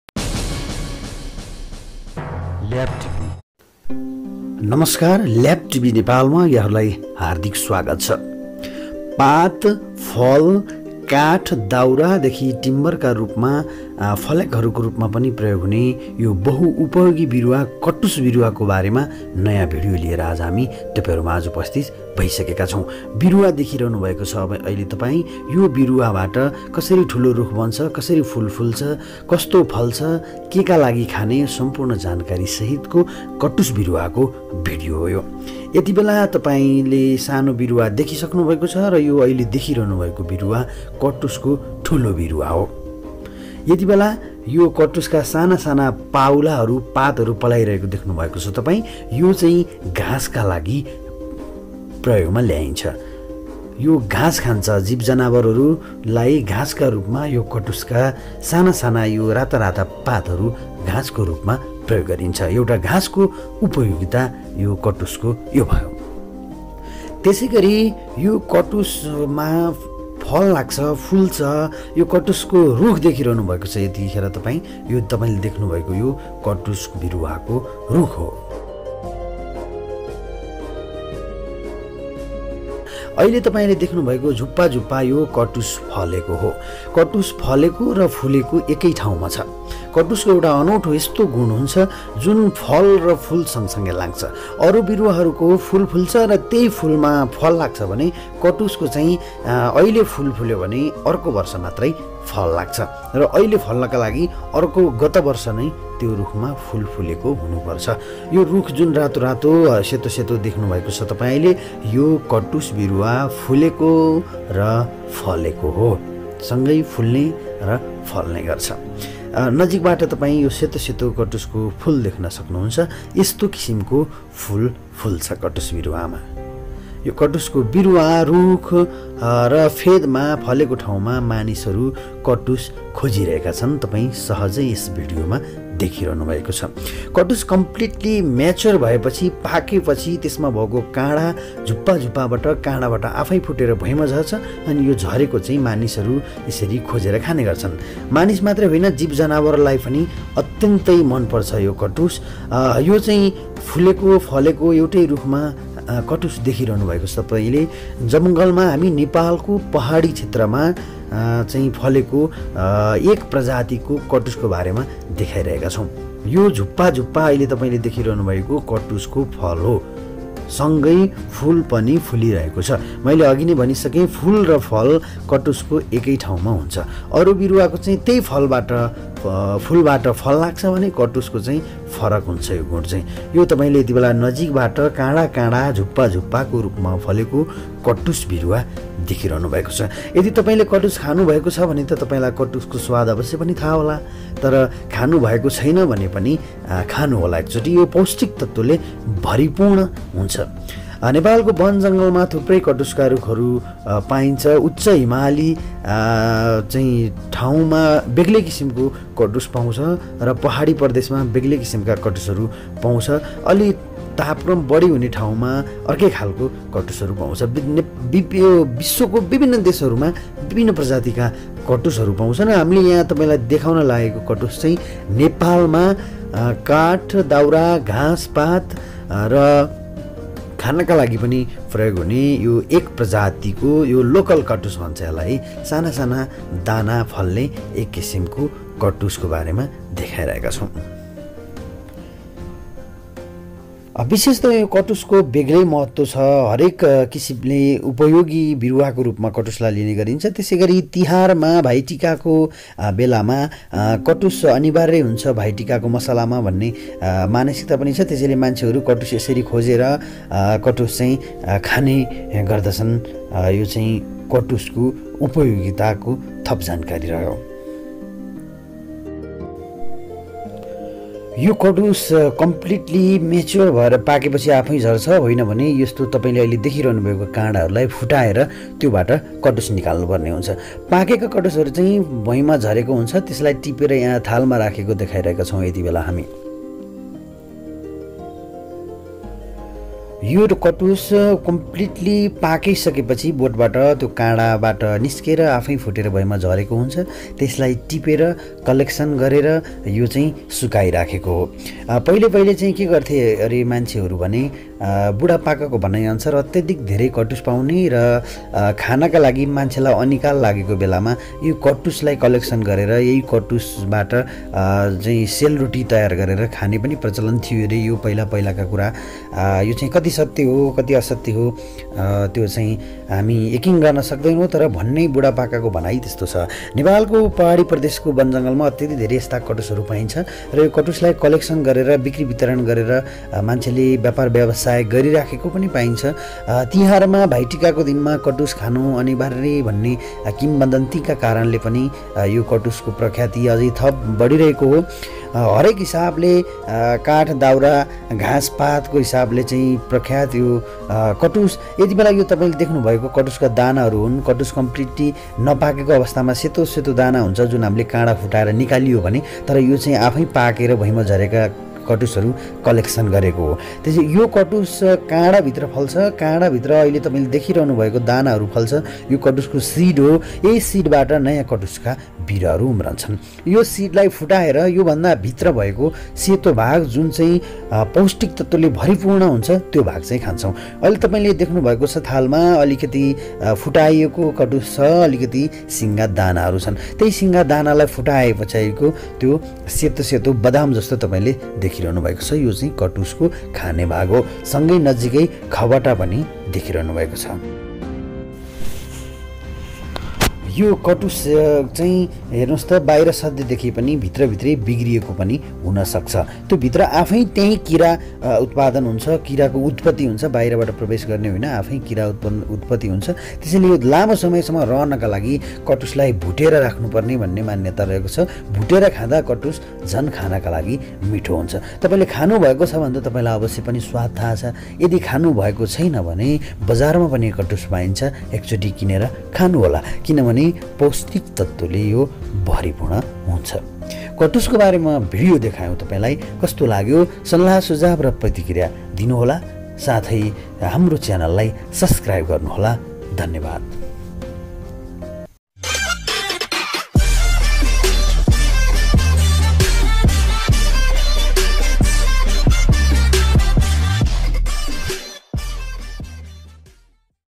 Castanopsis | KATUS | कटुस | पुरै जानकारि | LAB TV New Nepali Documentary video of Plant Kingdom
This is the documentary video of castanopsis plant. In Nepla it is known as Katus palnt. It is very useful grenn plant. The leaf of this plant is use for grass for animals like goat, cow, buffallow etc. and hard part of this castanopsis plant is use for timber and fire wood.Castanopsis plant is also ues for medicine. The seed of this plant is use for consumption. The inner part of seed of katus plant is very testy and fulfil of nutrients.Many species of Castronapsis plant is found all over the world. This video is of local family of castanopsis that found in Nepal.